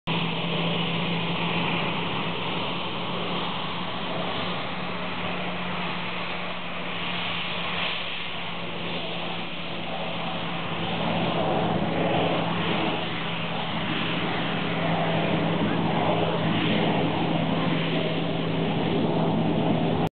Zivilflugplatz Nordholz Gulfstream G550 · DC Aviation
Eine  Gulfstream G550 · DC Aviation zu Gast auf dem Zivilflugplatz Nordholz. Die Maschine war die D-ADCA